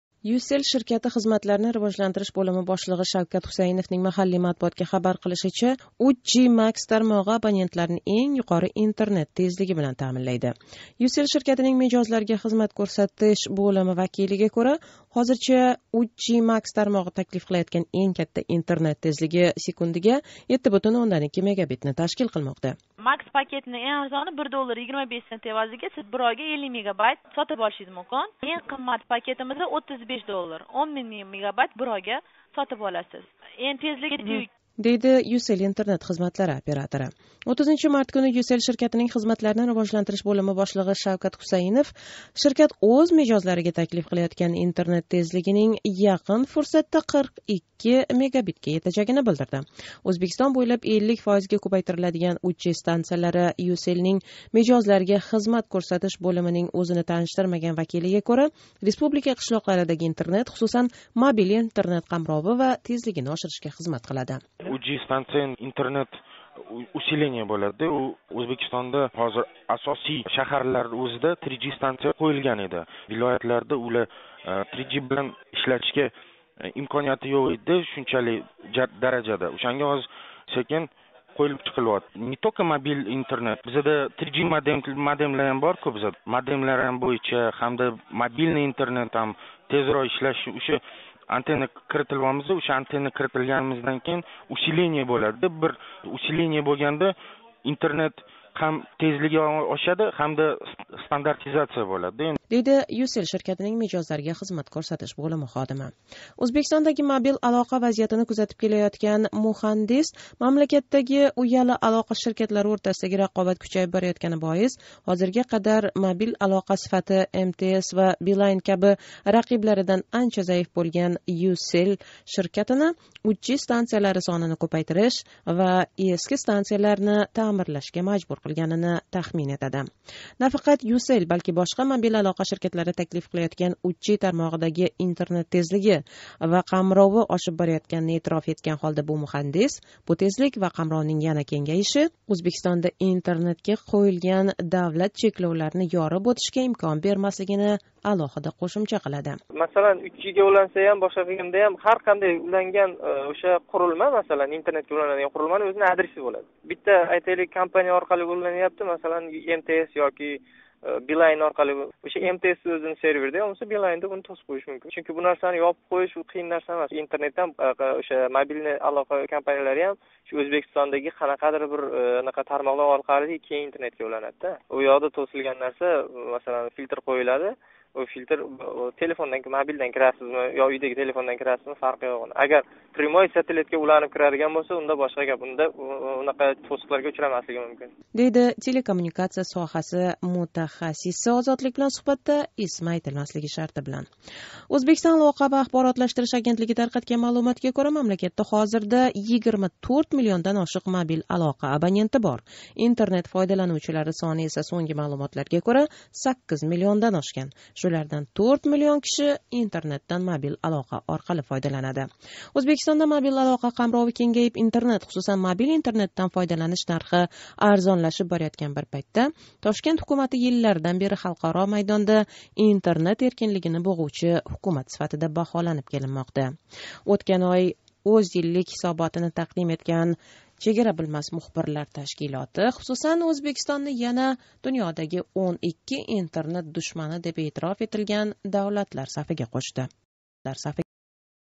Uzbek Ucell Интернет тезлиги ва қамровини кучайтирмоқчи
Ўзбекистонда МТСдан кейин иккинчи йирик алоқа оператори ҳисобланган Ucell ширкати республикадаги 3G станциялари сонини 50 фоизга кўпайтиришни режаламоқда.